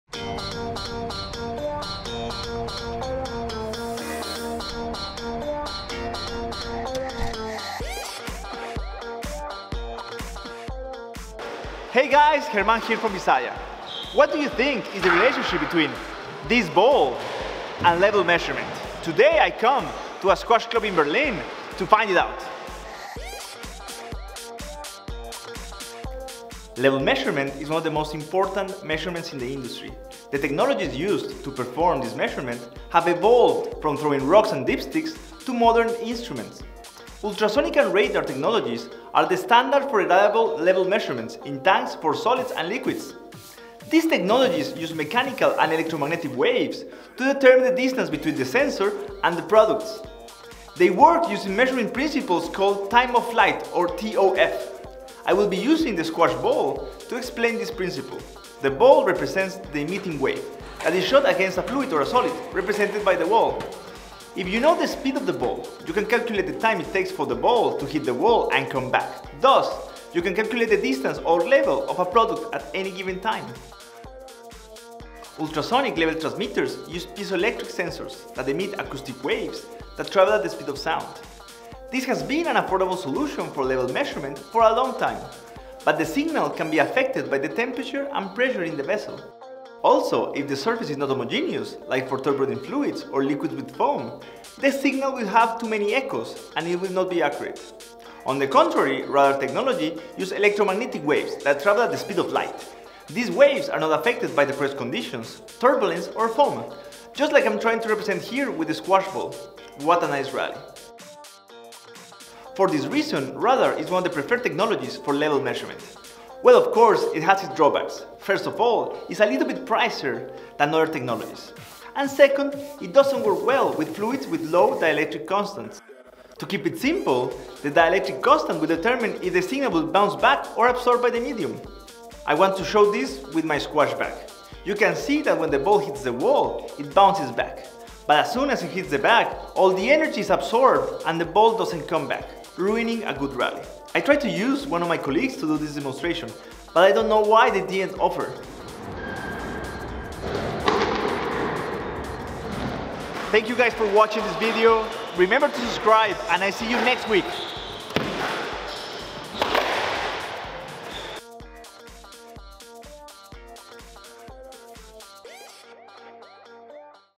Hitting it up with level measurement. Visaya Weekly Episode 13.
Visaya is now Automation24.

Hey guys German here again from Visaya, this week I'm visiting a squash club in Berlin to find out how we can hit it up with level measurement.

One of the most important measurements in the industry is level measurement. Ultrasonic and radar technologies are the standard for reliable level measurement.

Time of Flight devices uses mechanical and electromagnetic waves to determine the distance between the sensors and products. What else can I say? Just warm-up stretch and get ready for this intense rally!